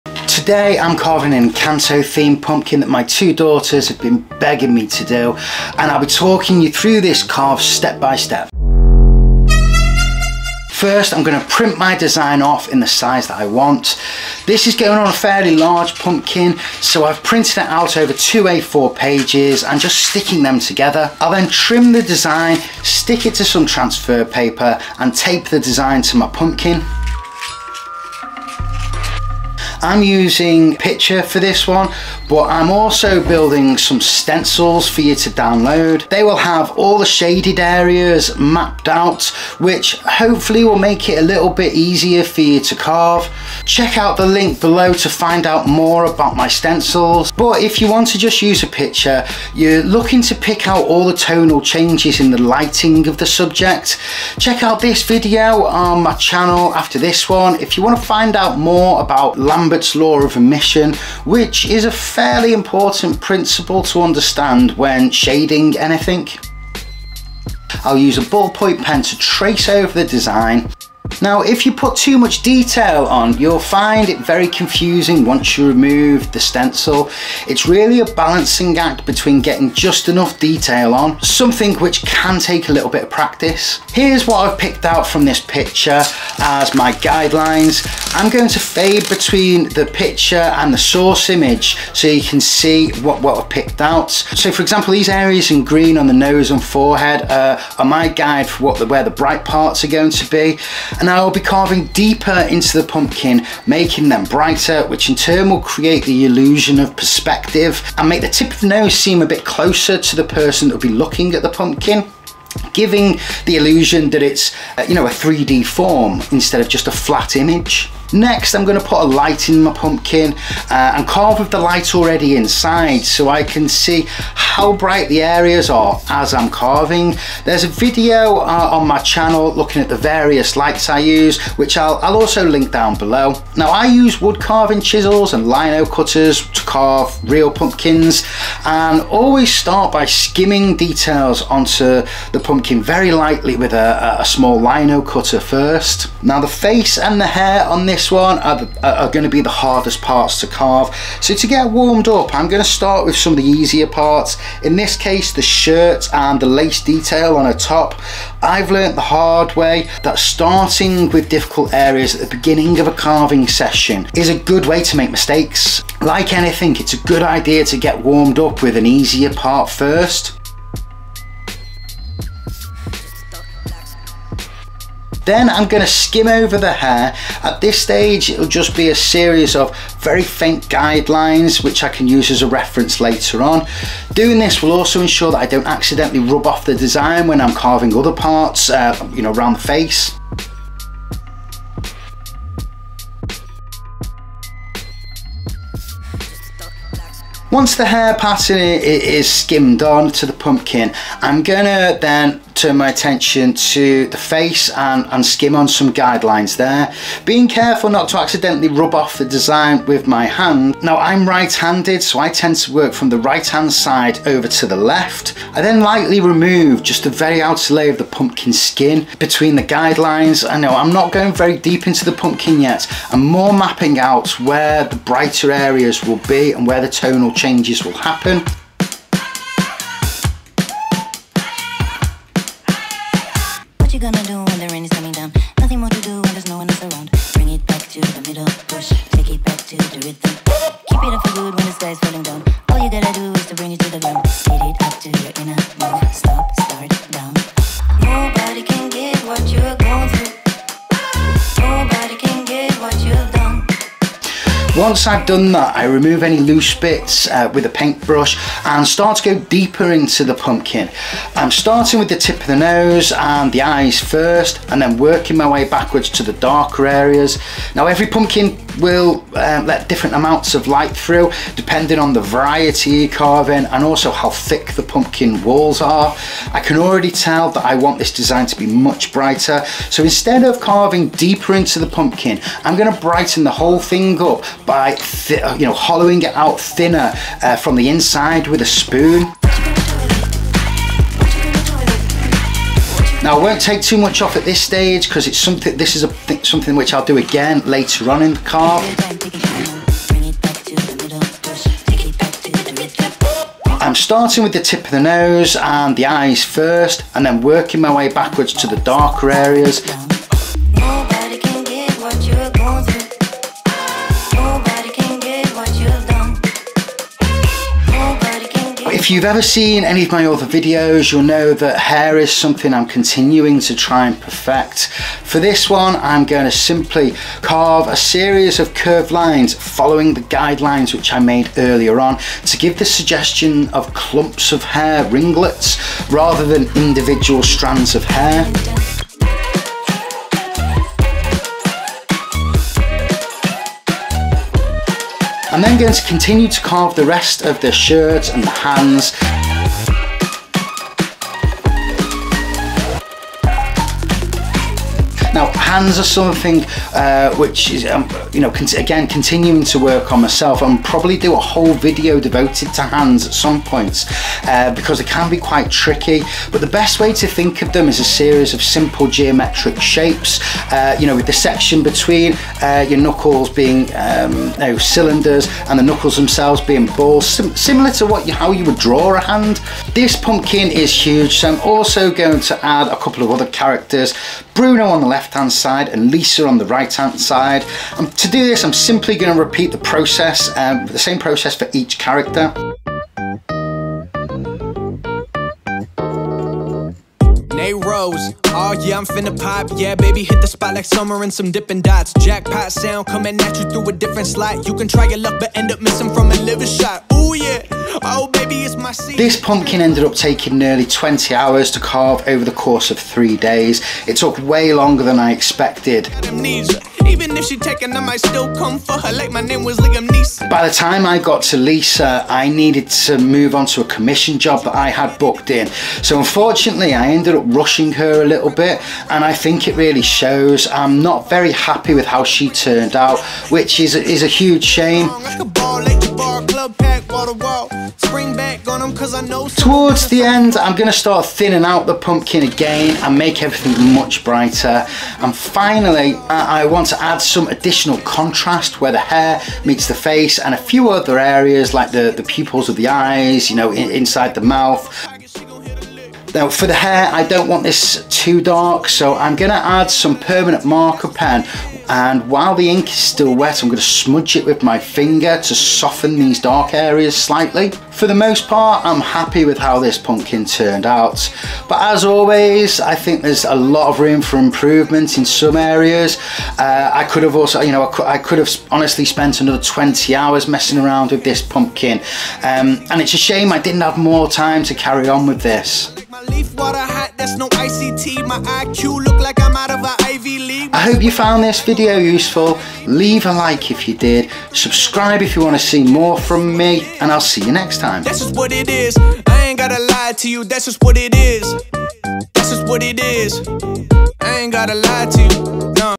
Encanto Pumpkin Carving Tutorial: Tips for Impressive Designs
Learn the art of pumpkin carving with this step-by-step tutorial featuring an Encanto-themed design. In this video, I'll guide you through the process of creating a stunning pumpkin masterpiece. From printing and transferring the design to carving techniques and lighting tips, you'll gain insights into every aspect of the pumpkin carving journey.
Discover how to achieve depth, perspective, and intricate details that bring your pumpkin to life.
Whether you're a beginner or looking to enhance your pumpkin carving skills, this video provides valuable techniques and tricks to make your creations stand out.